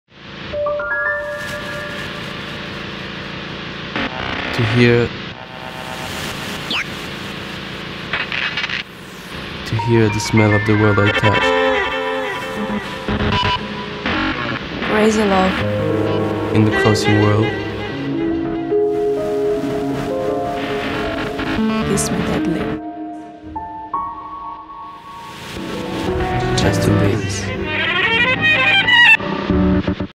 JUSTIN DAVIS 2019 Spring Summer Campaign Movie『KISS ME DEADLY』
ジャスティン デイビスの新作は「KISS ME DEADLY」をテーマに 1970 年代後半の 初期パンクからインスパイアされた自由なスピリットと情熱的なニューコレクション。